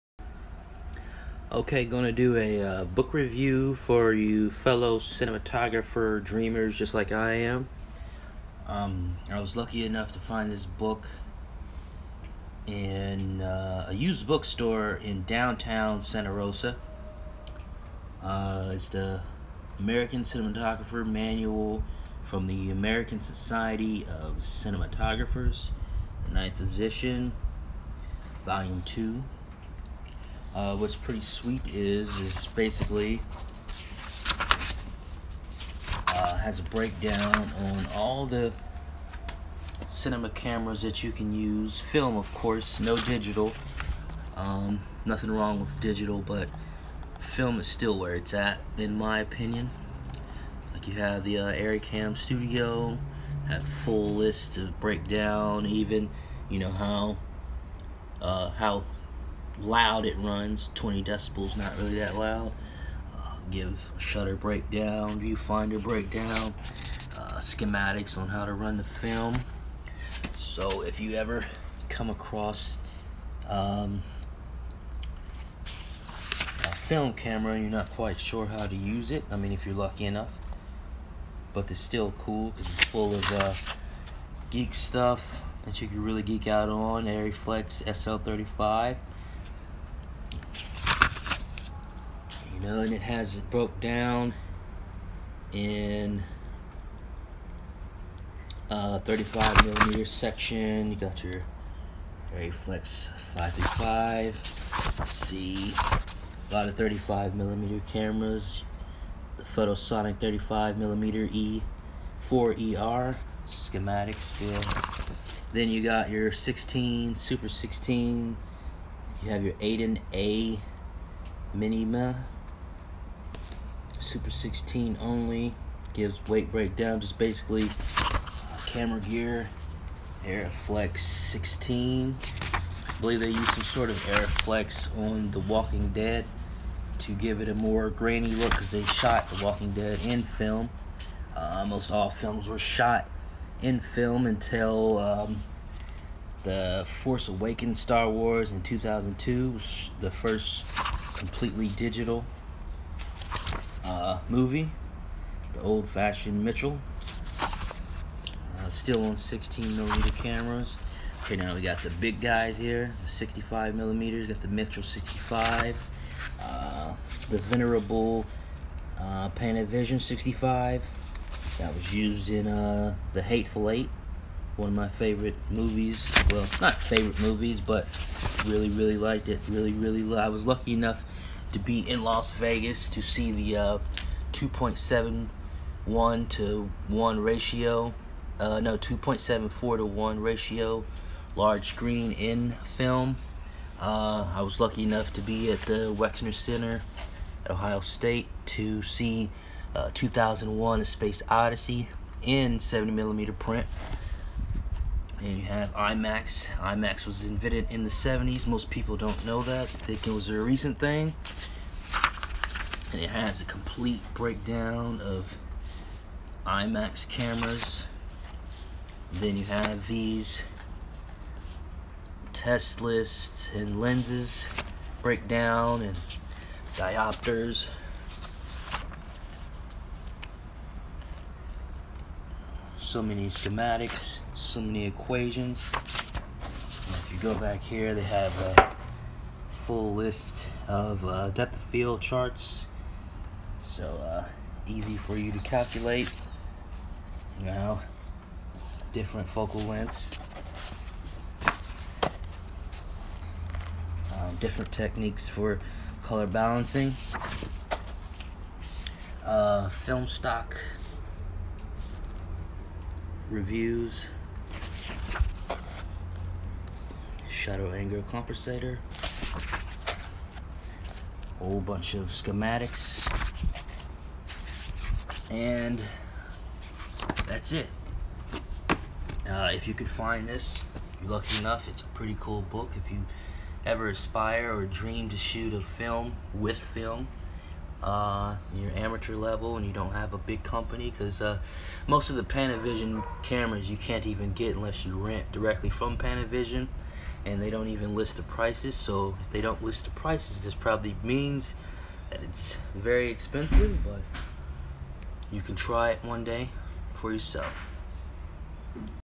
American Cinematographer Manual used book review
Used bookstore find shot with filmic pro app
American society of cinematographers